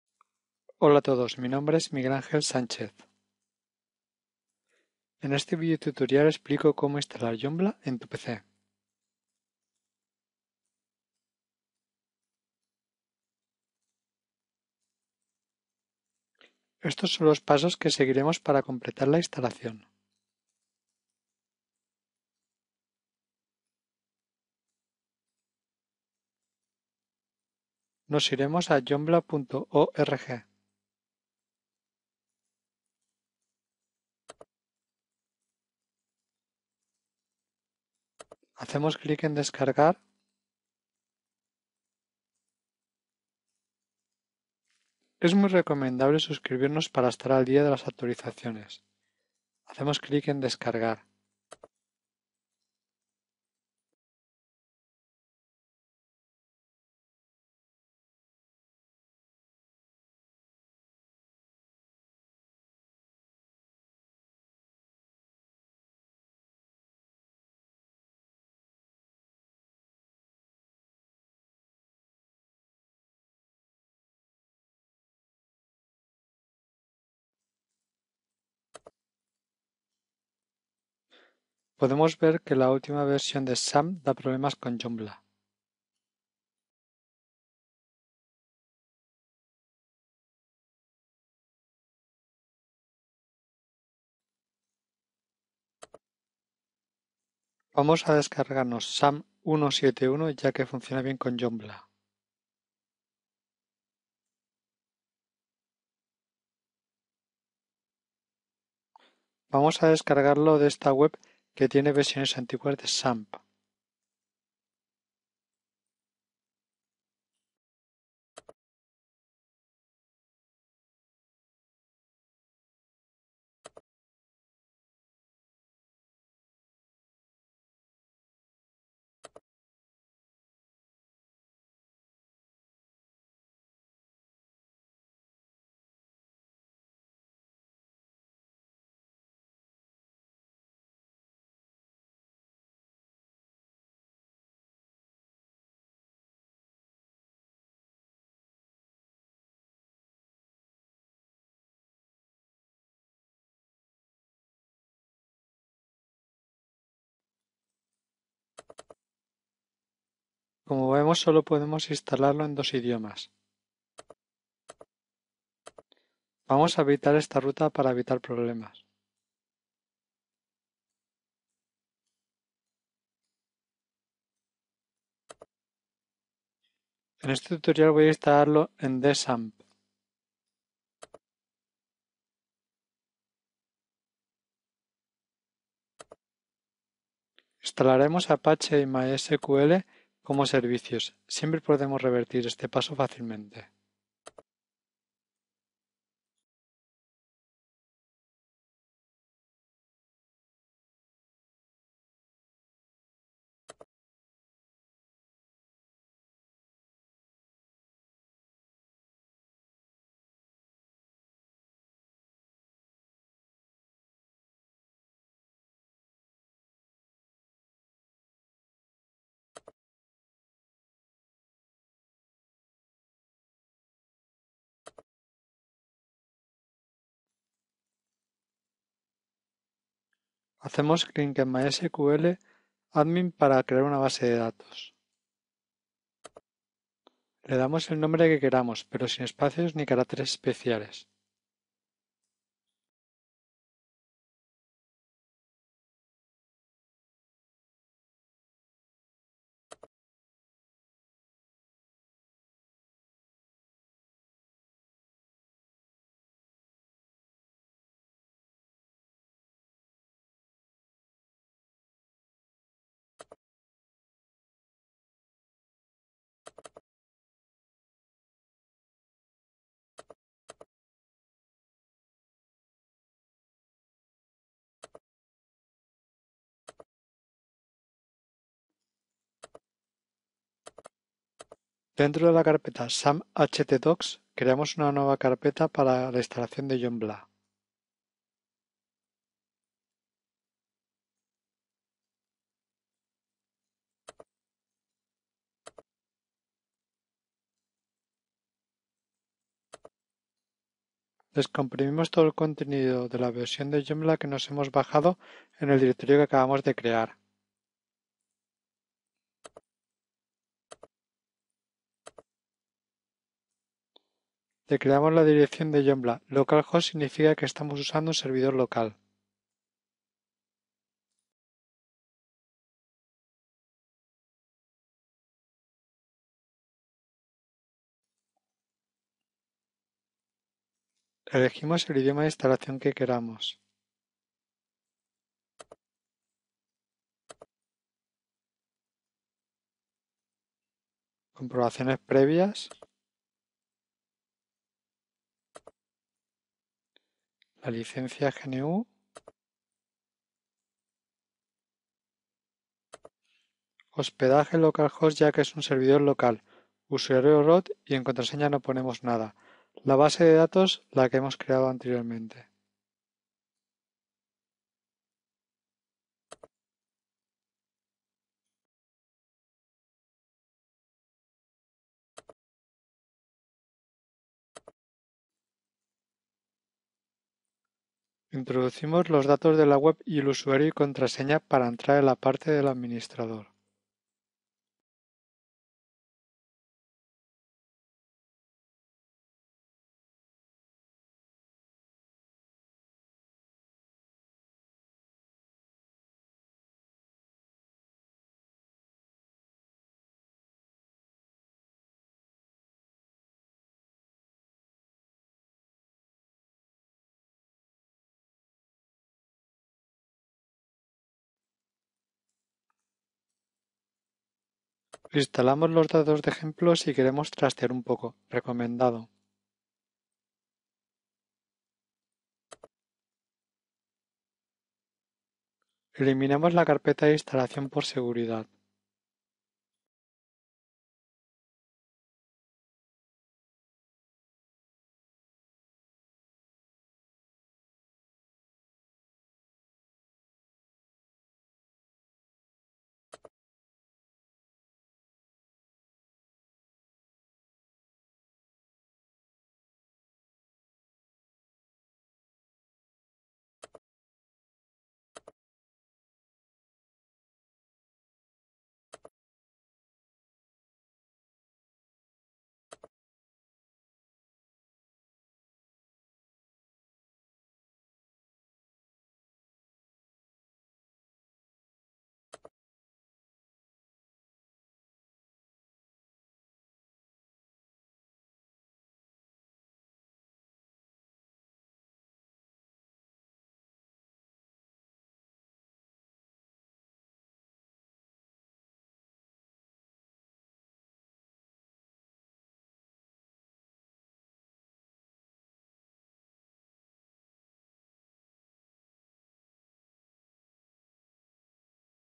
Tutorial de como instalar Joomla local (en tu PC) con XAMPP
Videotutorial que explica como instalar Joomla en un servidor local XAMPP 1.7.1